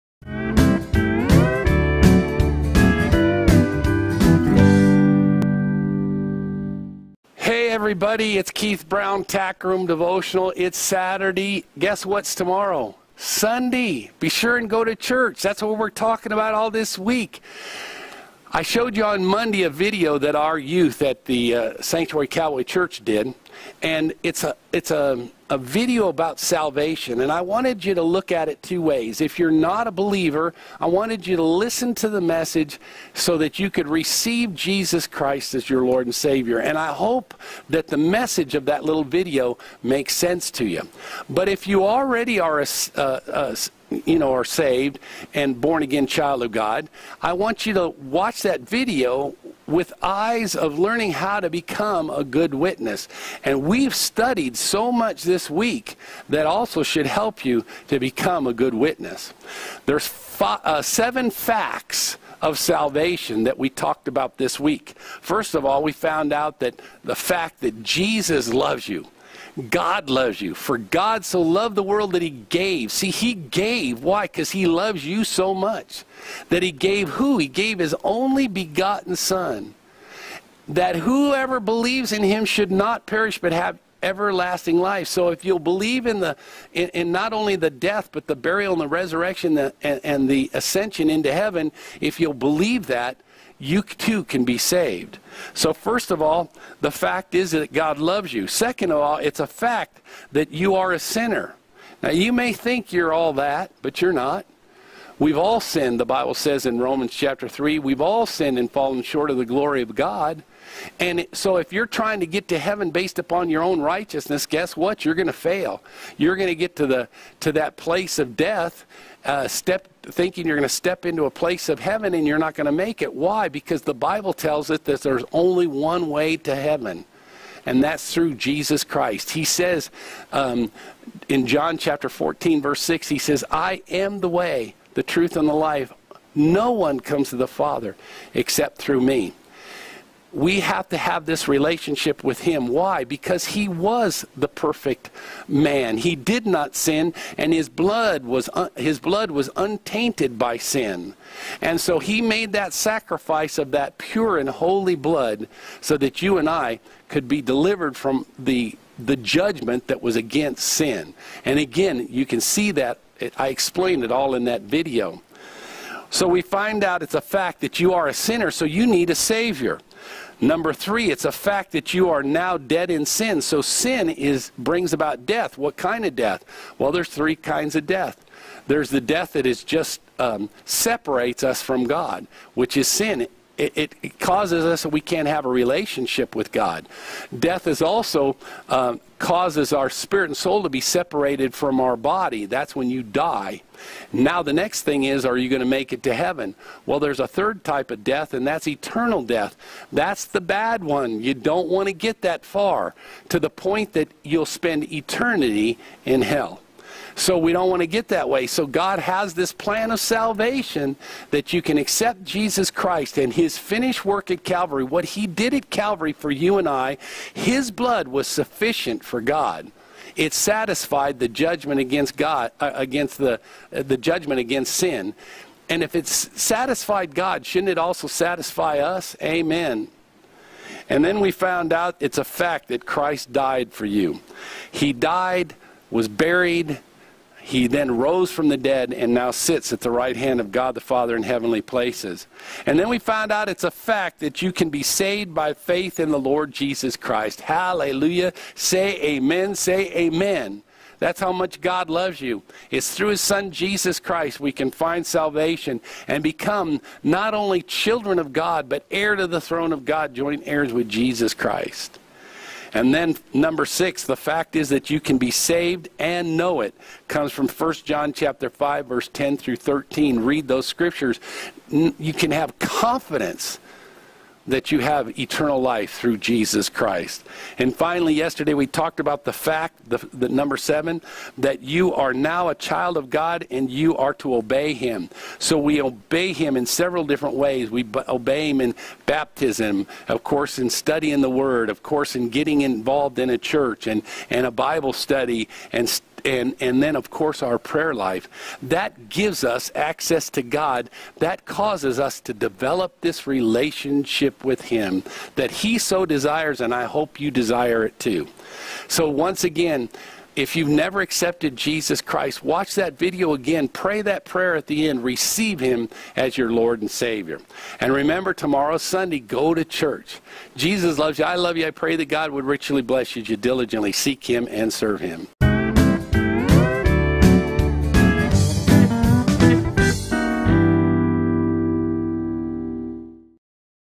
7/13/2019 God's Plan Of Salvation...ReCap
This is a recap of this weeks message and scriptures.
If you missed any, review the previous videos from this week
for details.